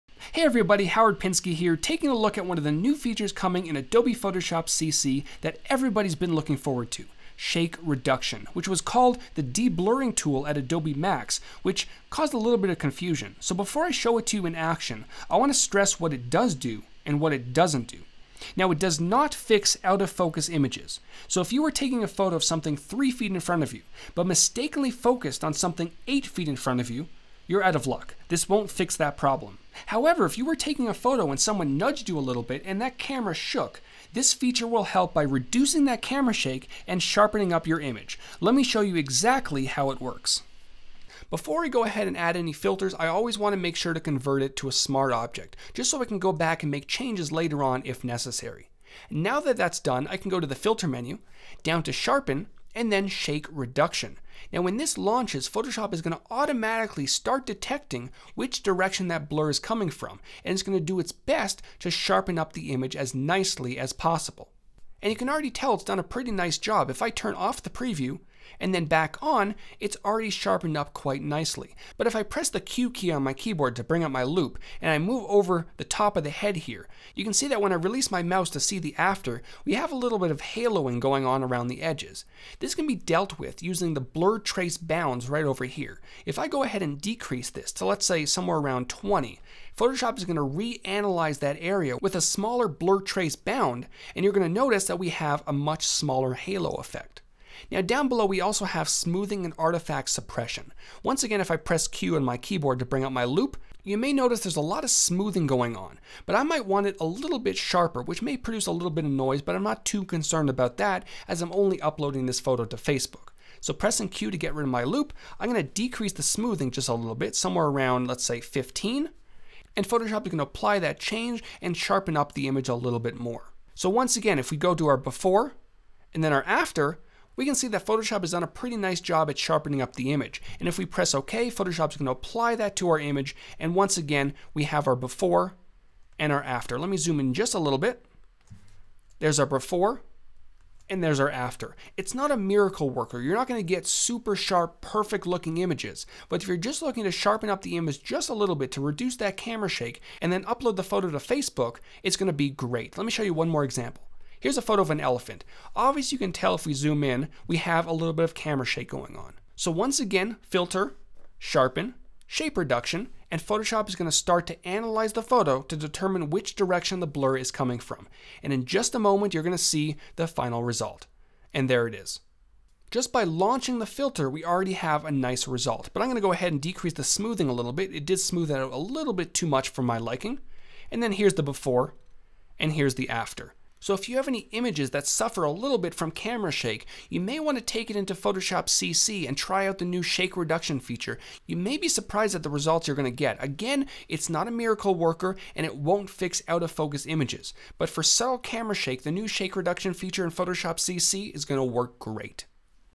How to Reduce Blur Using Shake Reduction in Photoshop
The new Shake Reduction feature in Adobe Photoshop CC will help reduce the camera shake of your images, and sharpen them up. ENHANCE! Photoshop CC will be released June 17th.